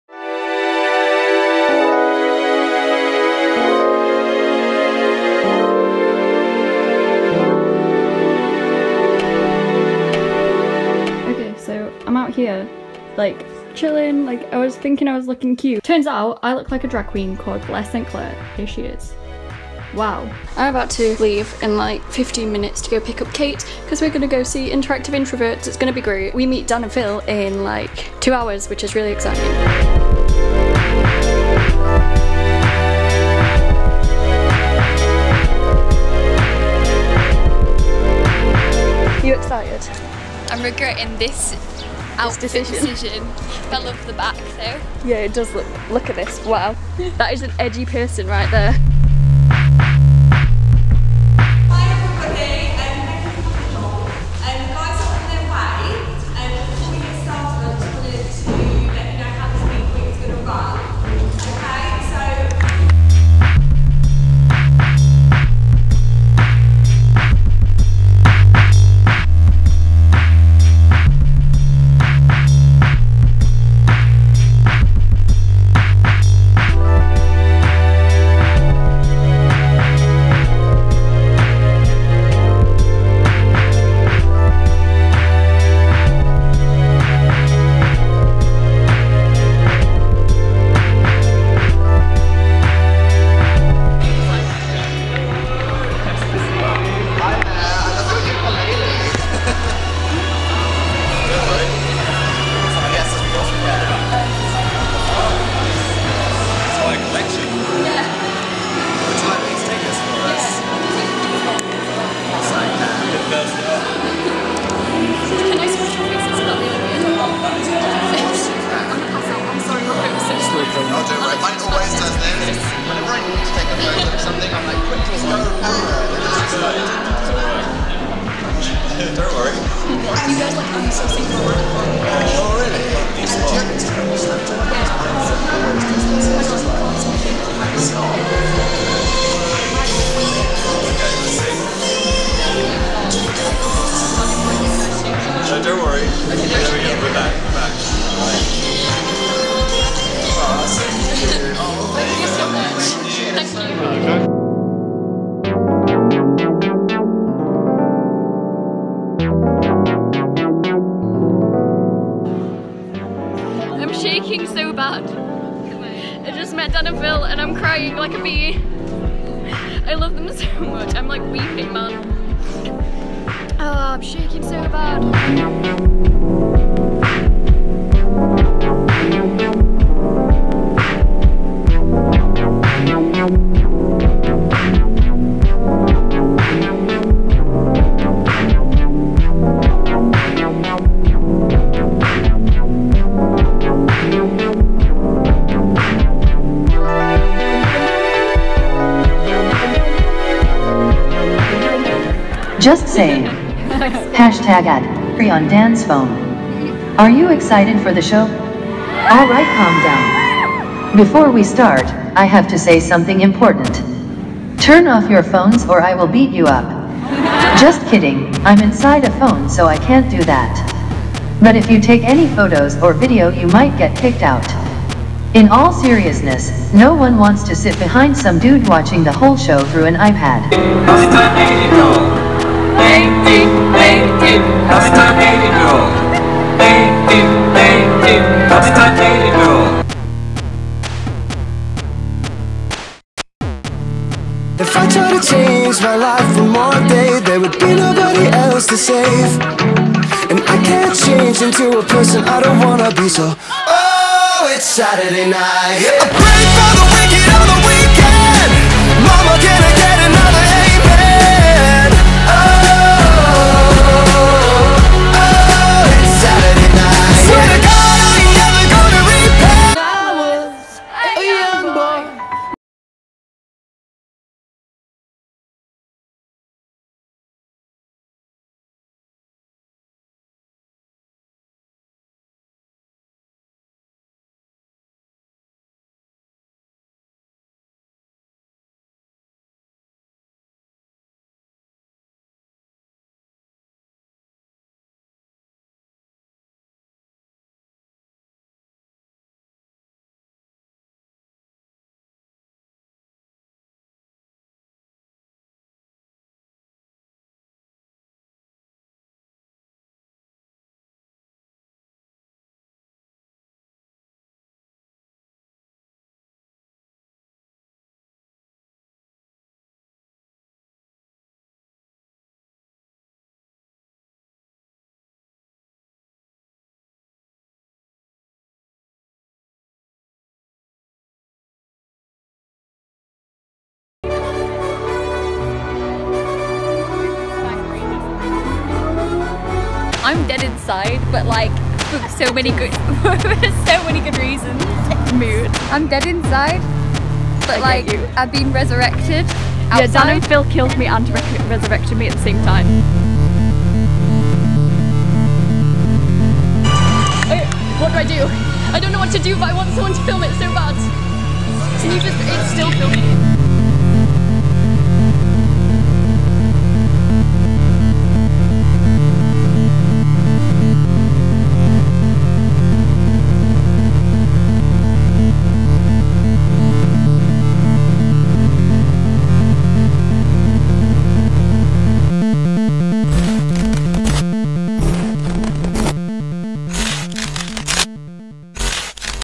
VIP INTERACTIVE INTROVERTS: I MET DAN AND PHIL
So I was lucky enough to get VIP tickets to interactive introverts on May 7th… I met Dan and Phil and was front row for the show. It was a great show and the boys were the nicest in the world. There is no spoilers for the show in this video but it was more just showing what VIP is like and some parts of the day. I feel like I shouldn’t have to say this but I am in no way bragging about it, I am so grateful that I got VIP tickets and can’t thank my mum and dad (they bought the tickets) enough. This video is to show what its like to meet Dan and Phil and to share the experience with anyone who can’t go...

ps. if the editing isn't as good on this video as other, im ill and its hard to concentrate

Ellielovesphan